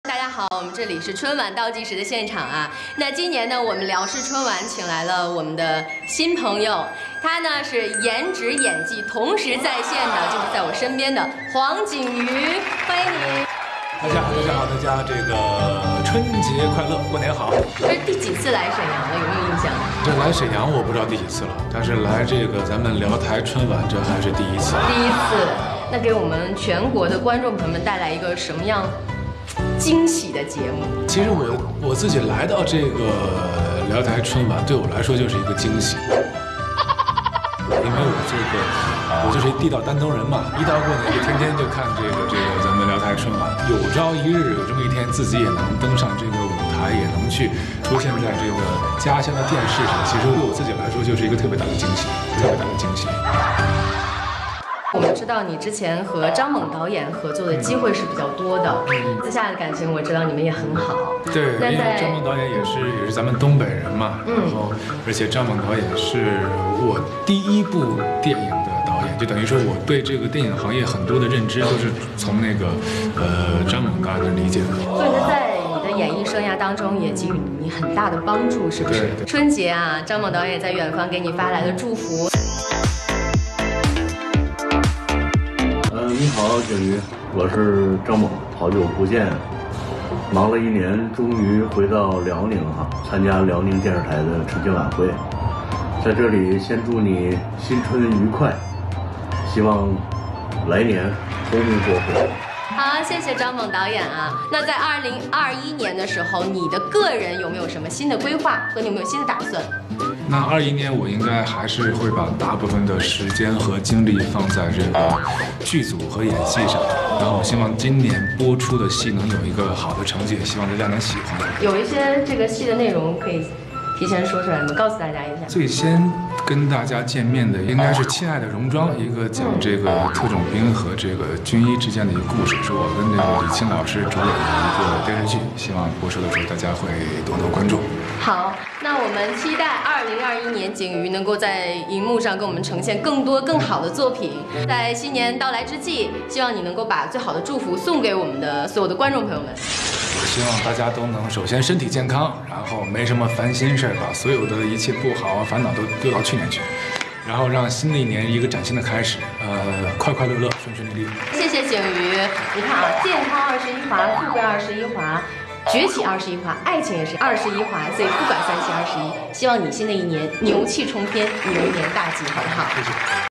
Johnny Huang Jing Yu - Liaoning TV Spring Festival Gala (interview 1)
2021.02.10 - Liaoning TV's interview with JohnnyHuang HuangJingYu before his performance on their Spring Festival Gala.